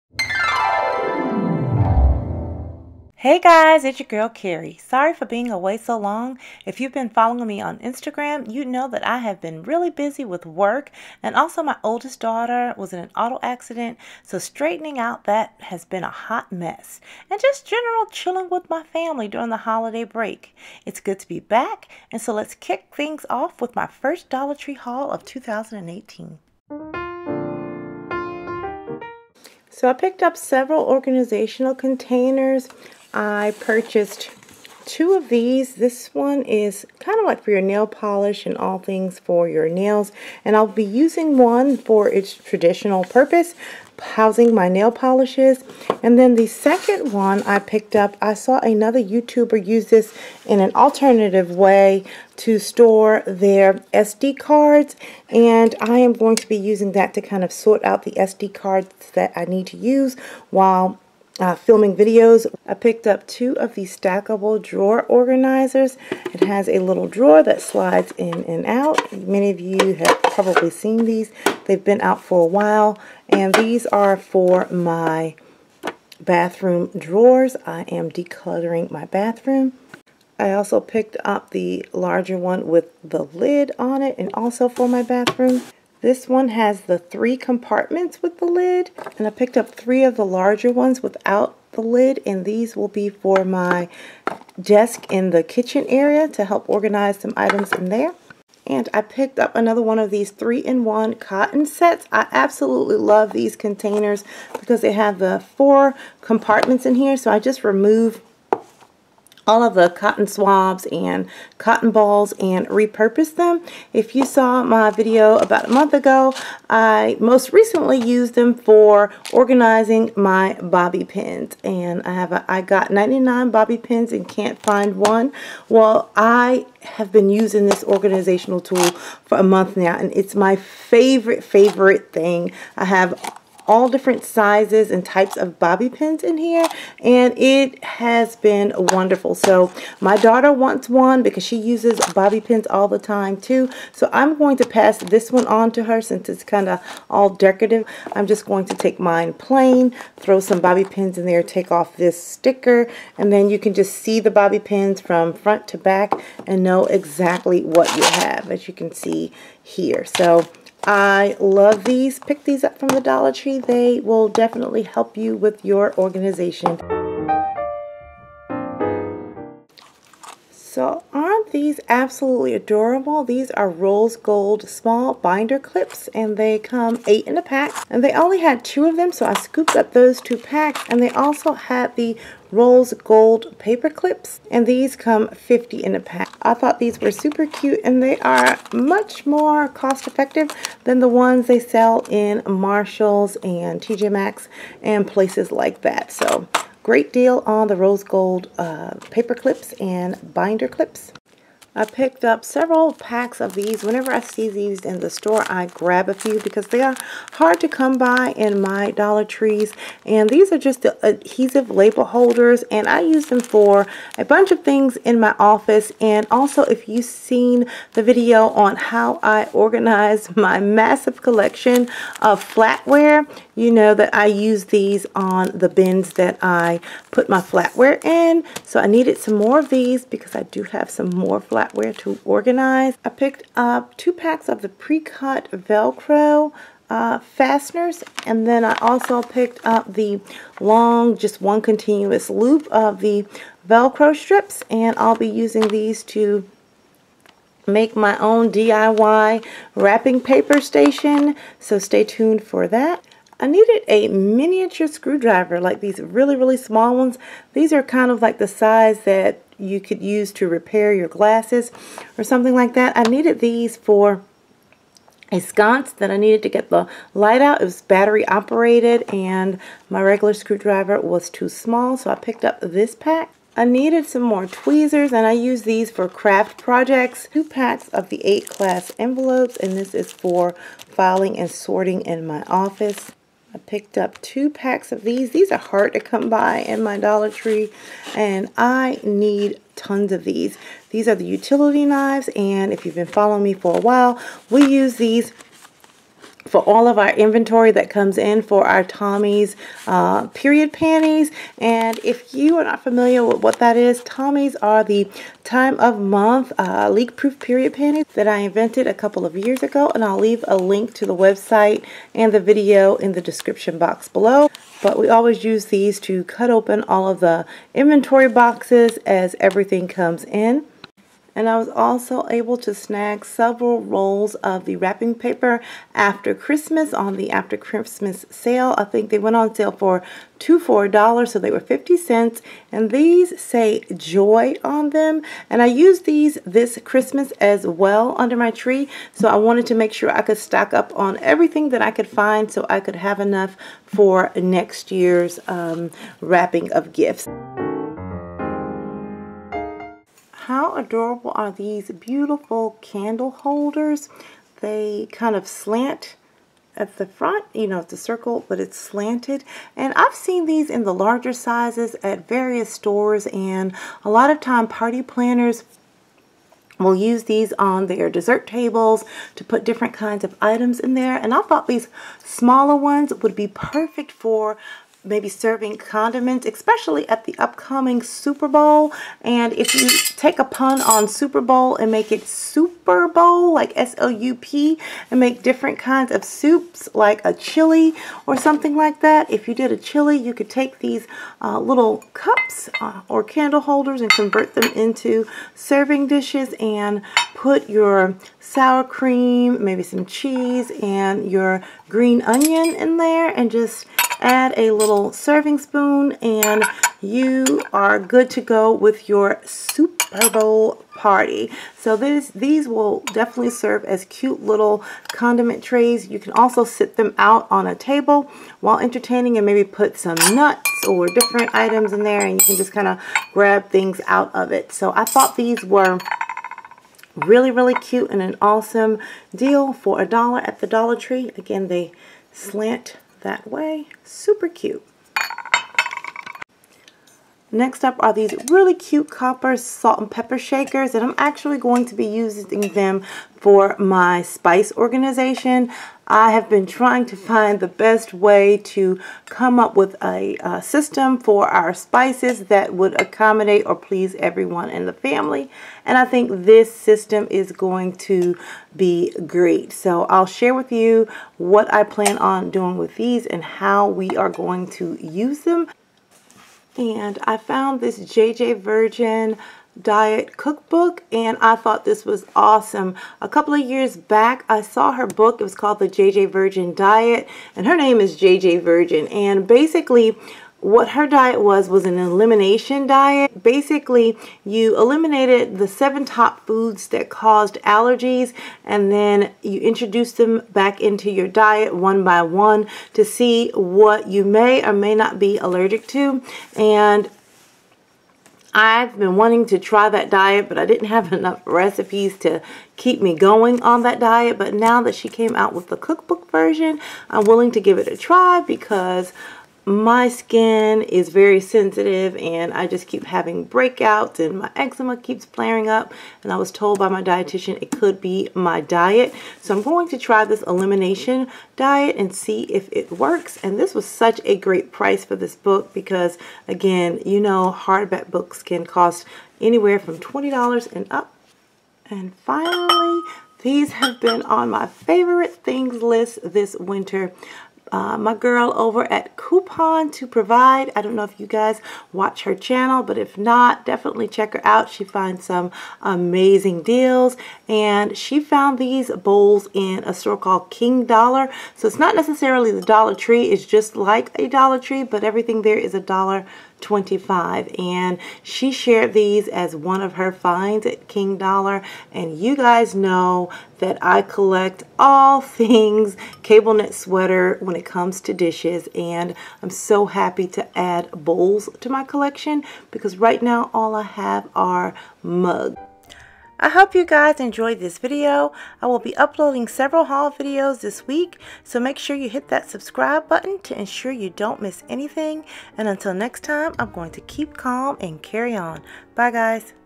Amazing Dollar Tree Haul 2018 New Finds, Organization, Entertaining, Rose Gold,
My first Dollar Tree Haul of 2018.  Great finds.  Enjoy!!